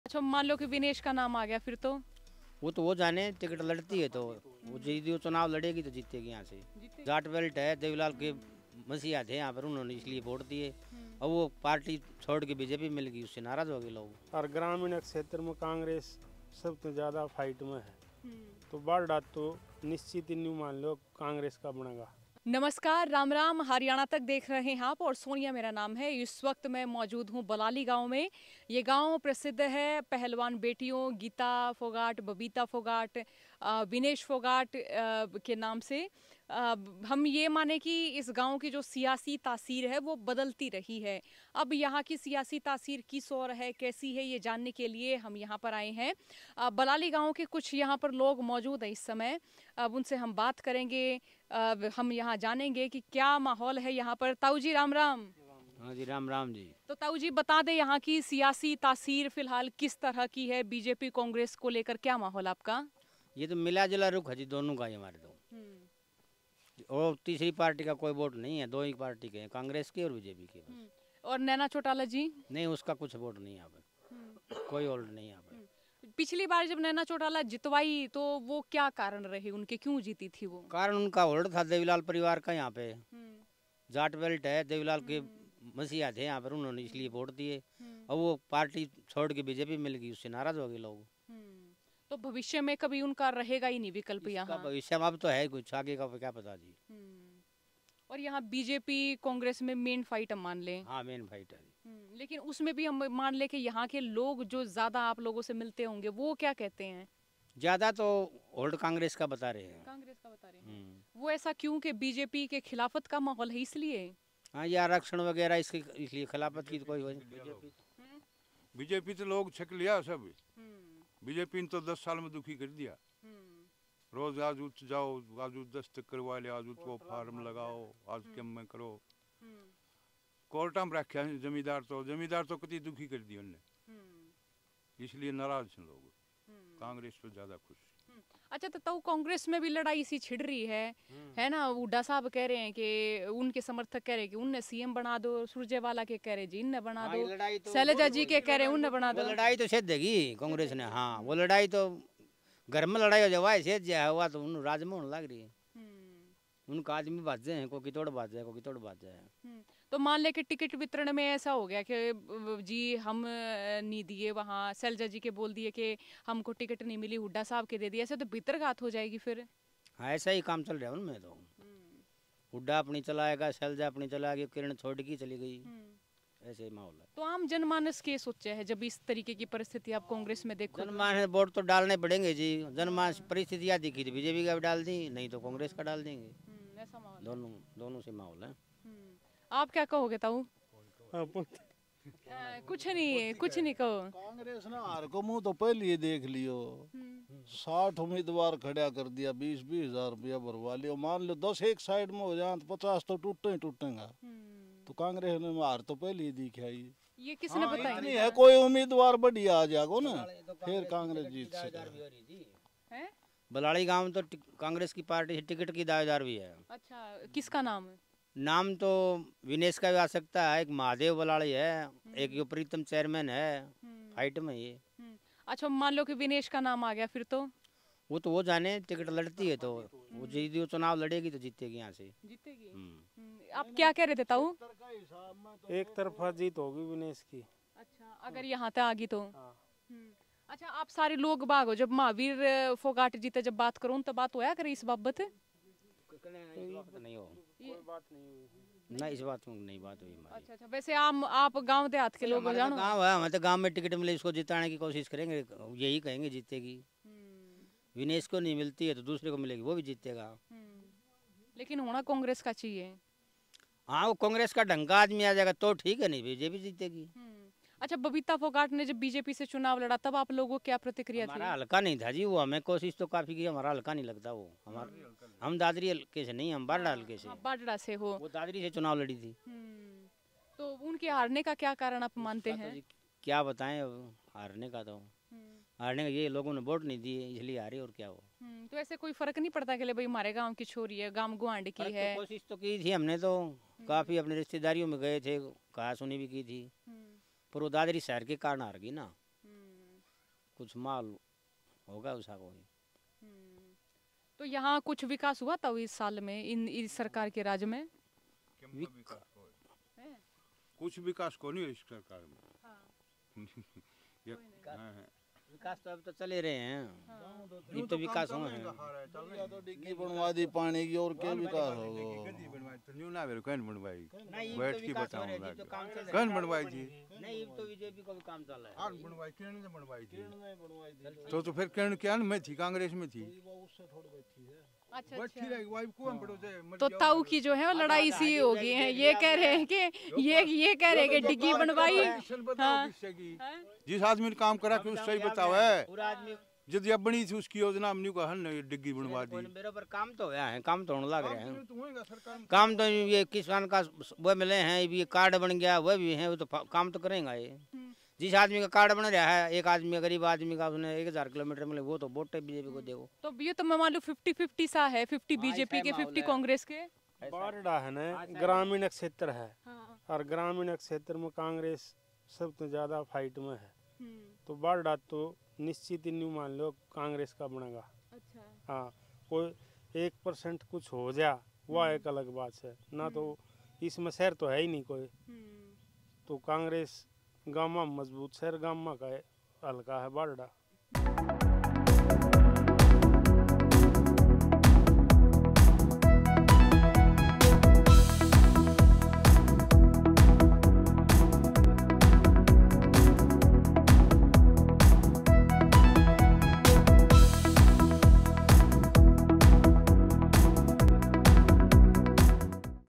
Haryana Vidhansabha Election :''या जाट बेल्ट सै''..Vinesh के चुनाव लड़ने पर बलाली में क्या माहौल? ।
Vinesh Phogat अगर दादरी से चुनाव लड़ती हैं तो एकतरफा जीत होगी.बाढड़ा से नैना चौटाला को वोट दिया था लेकिन वो इन सालों के दौरान आईं नहीं तो उनका जनाधार नहीं बचा.यहां बीजेपी-कांग्रेस में ही फाइट होगी.हुड्डा-सैलजा अपनी-अपनी चलाएंगे तो उन्हें नुकसान होगा- फोगाट बहनों के गांव बलाली के लोग बोले.